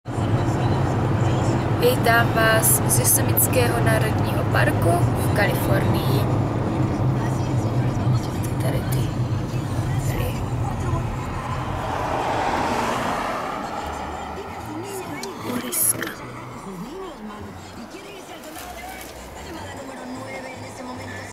Národní park Yosemite
Cesta autem národním parkem Yosemite. Více informací v článku